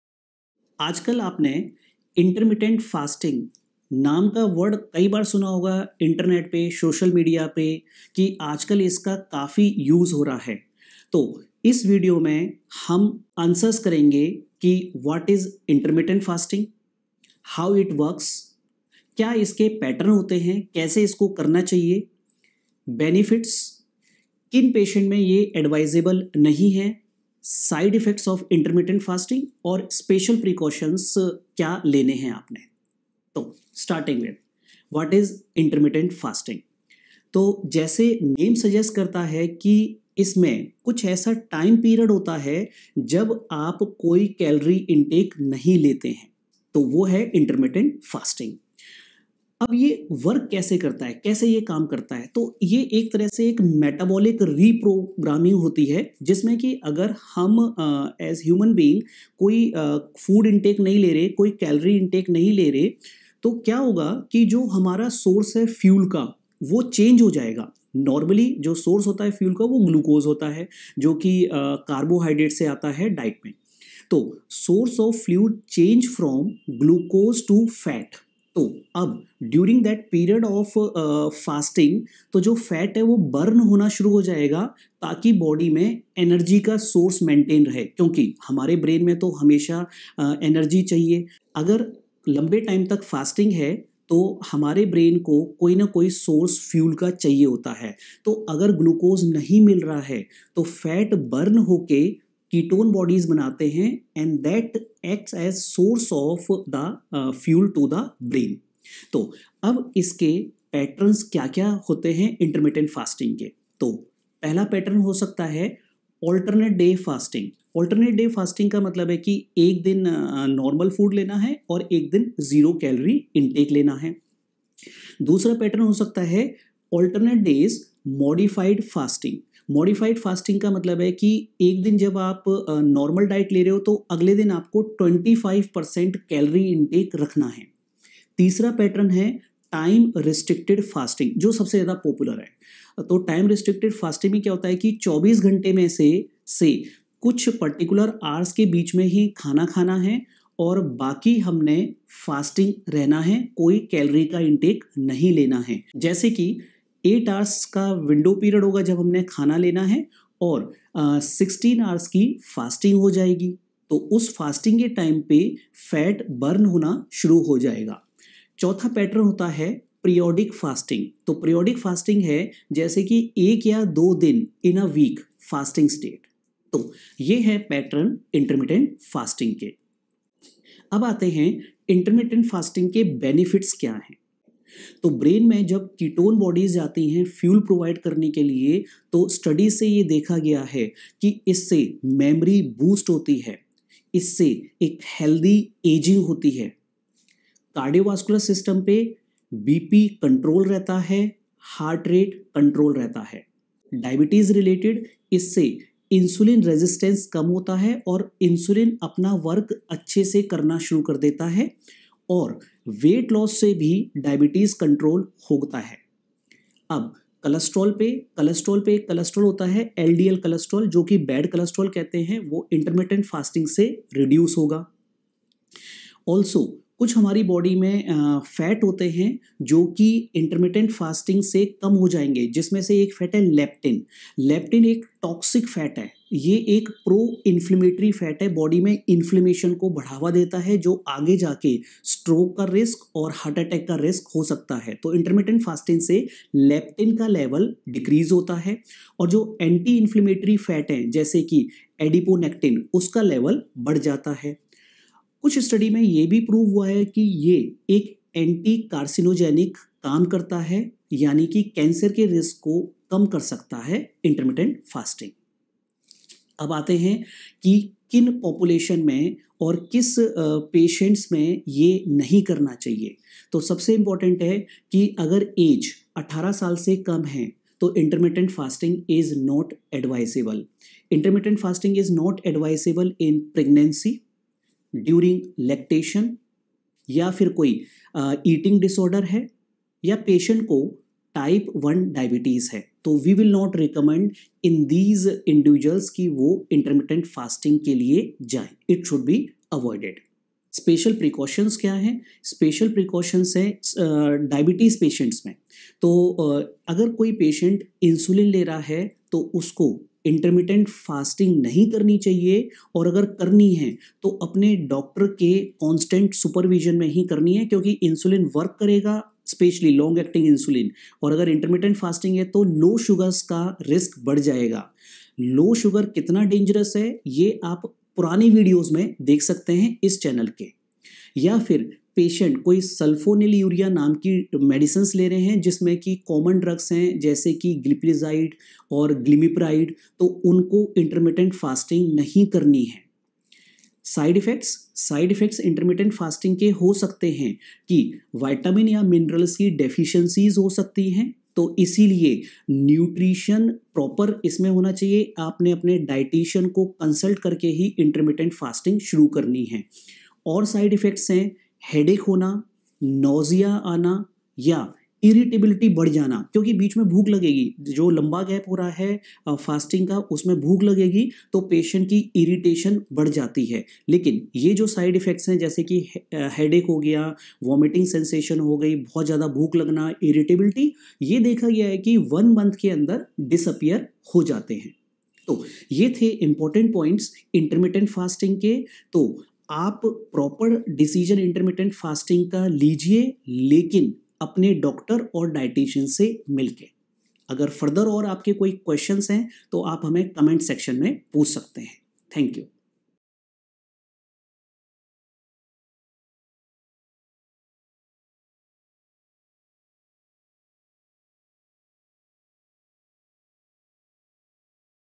Truth About Intermittent Fasting
what is intermittent fasting?How it is done?Side effects? Who should avoid it? All these questions are answered in this video.